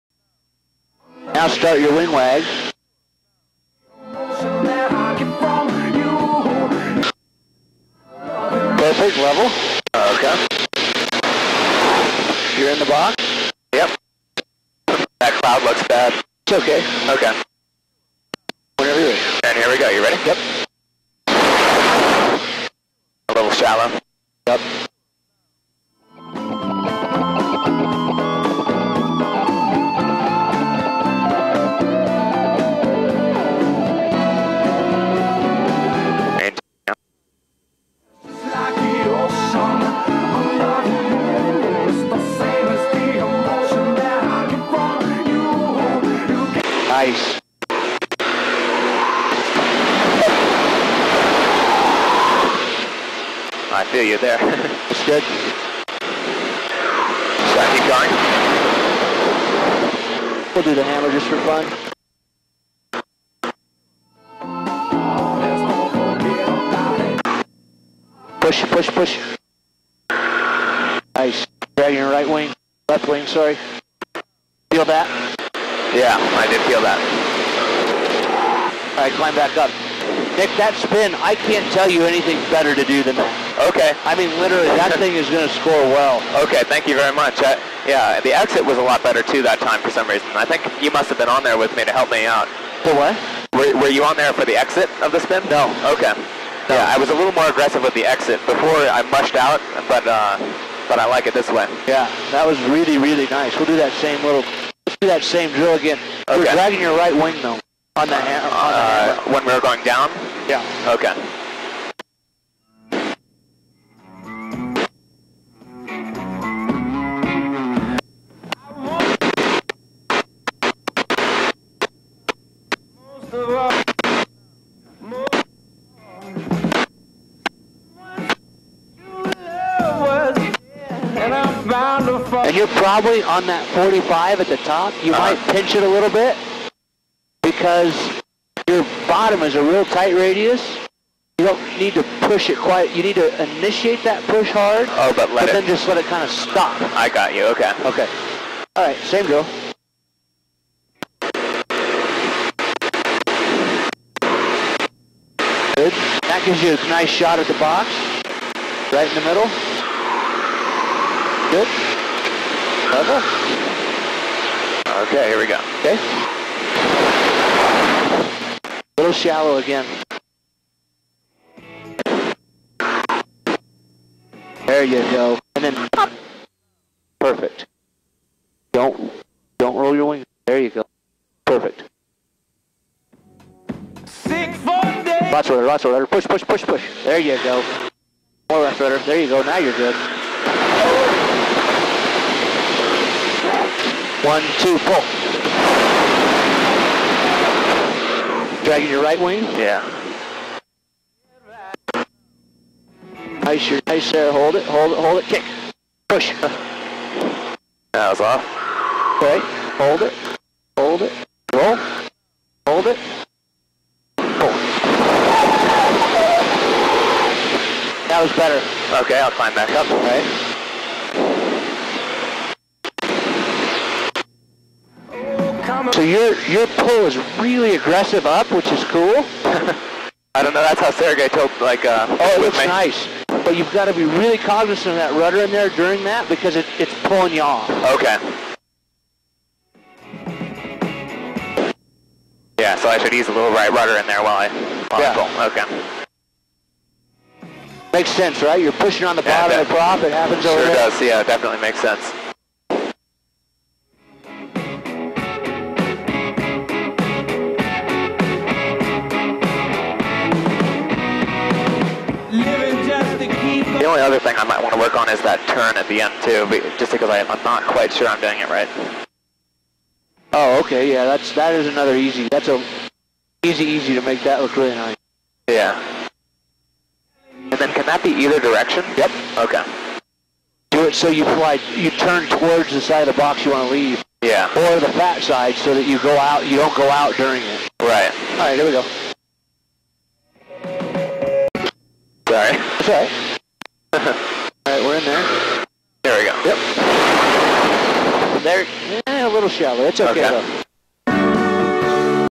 Nick & Chad 1
Found this old video of Nick & Chad practicing the opening of the Sportsman